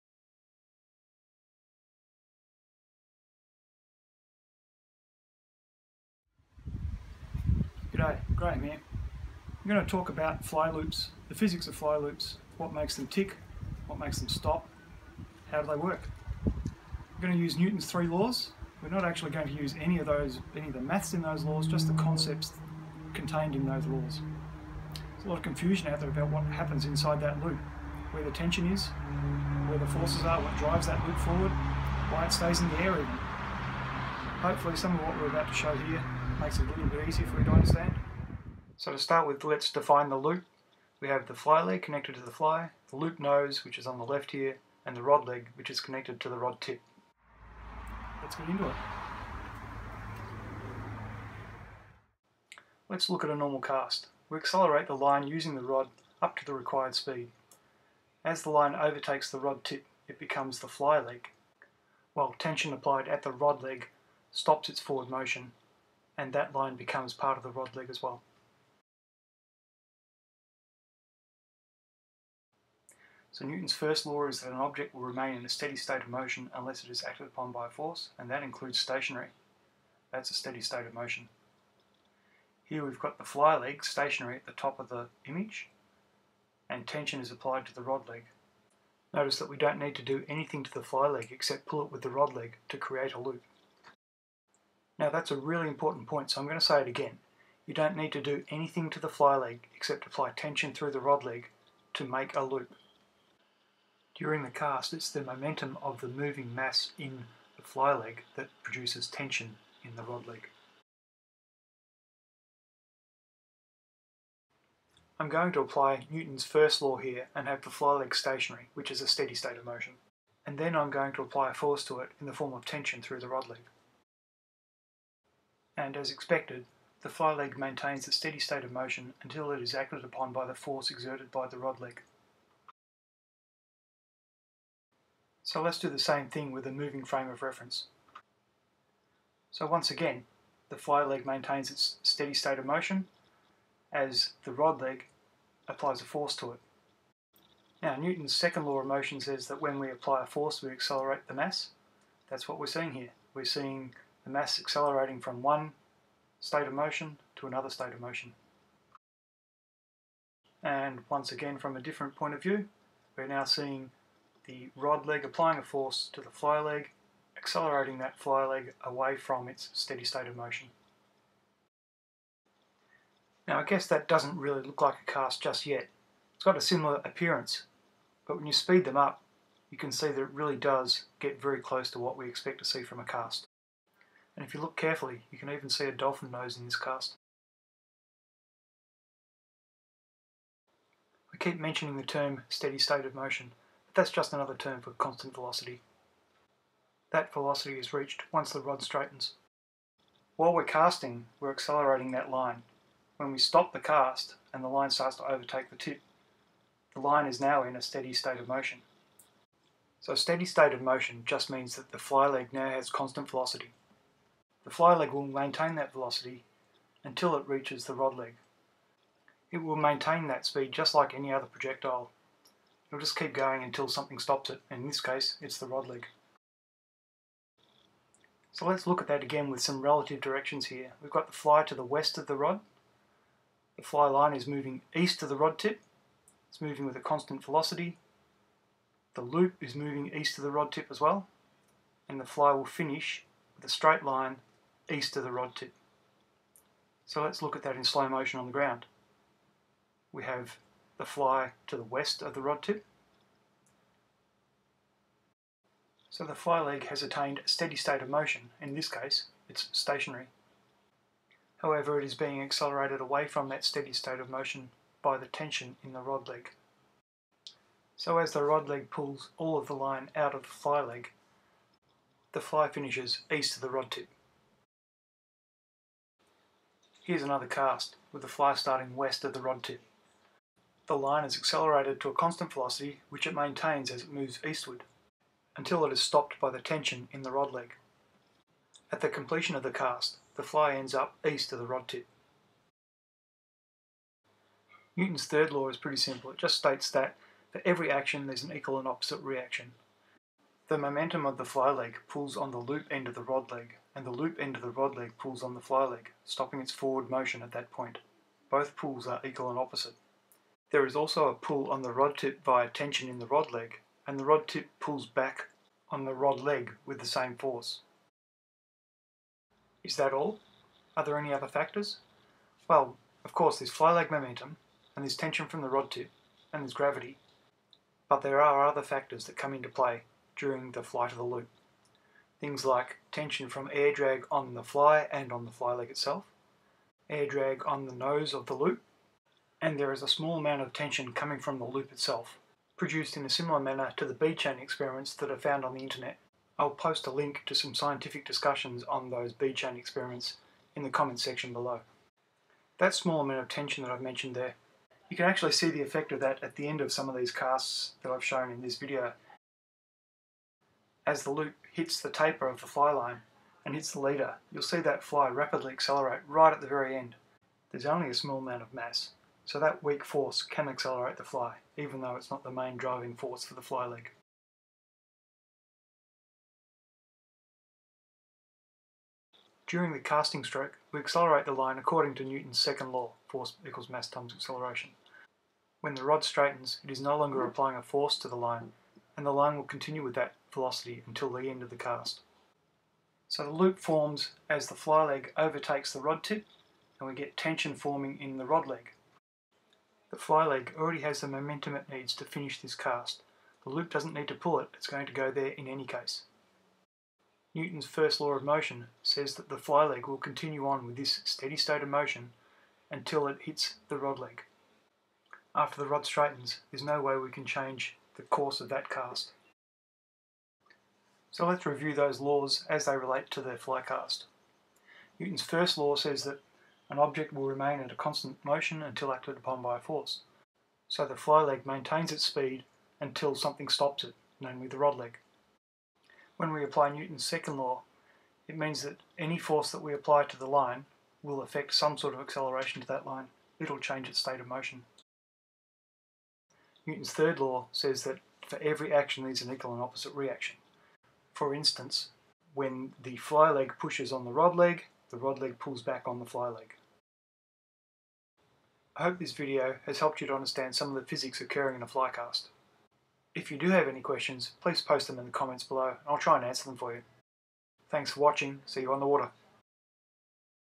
Newton's Laws Motion and The Fly Cast
This (updated) video is a discussion on how Newton's Laws of Motion apply to the fly cast. My sincere thanks to Walter, Grunde and Vince for reviewing the video over several editions.

There is an article in Scientific American about the findings of a scientific paper on the bead chain experiment which is a little bit easier to read than the paper. This is the article